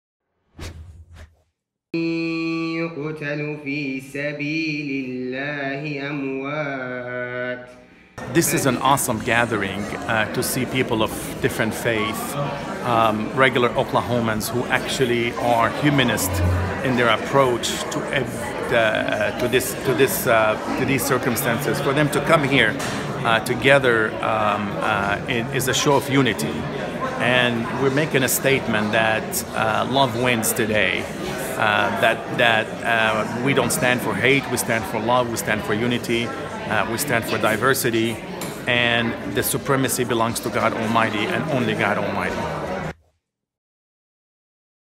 Metro faith community gathers for NZ prayer vigil
The prayer service drew a standing-room-only crowd of about 400 to show solidarity in the face of the New Zealand terror attacks.